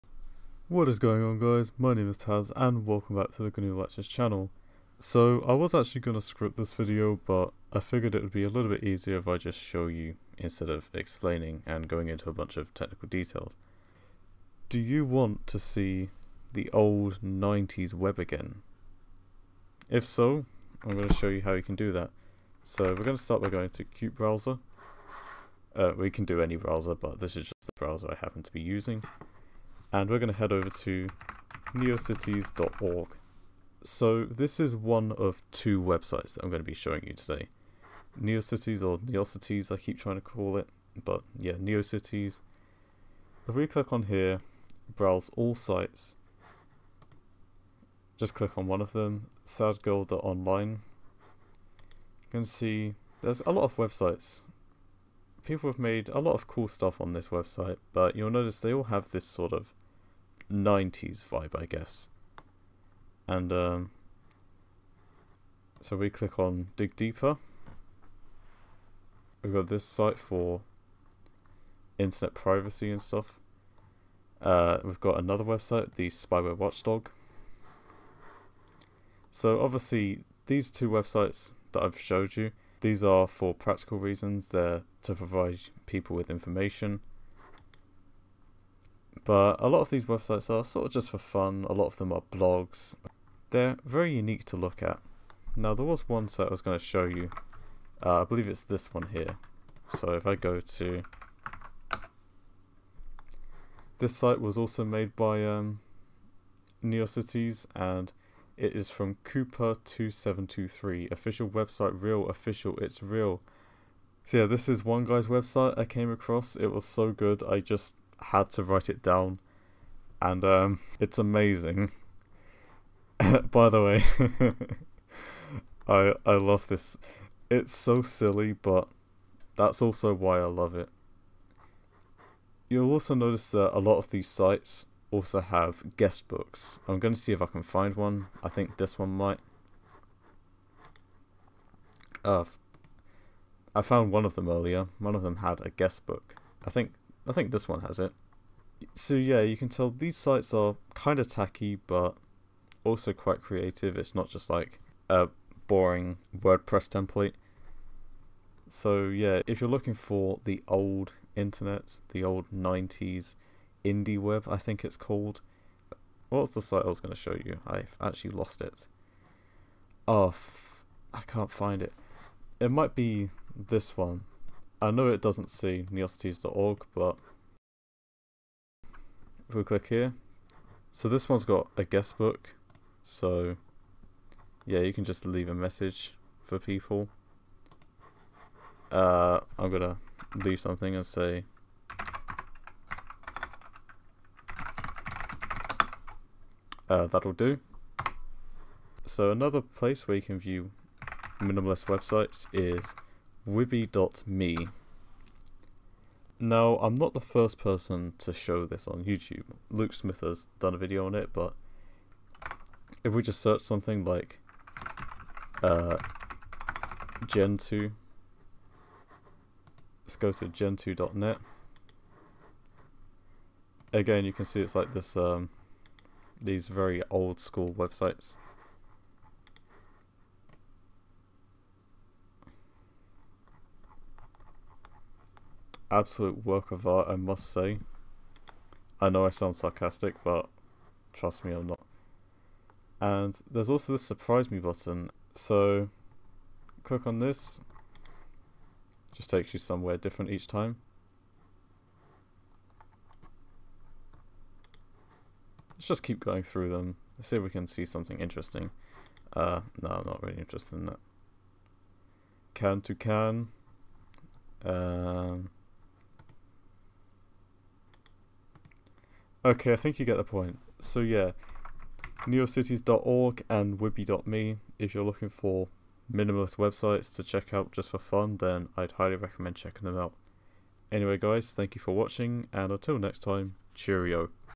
An informal look at old school websites
Just me browsing neocities.org and wiby.me. Apologies for any cuts or stuttering, this video was not scripted.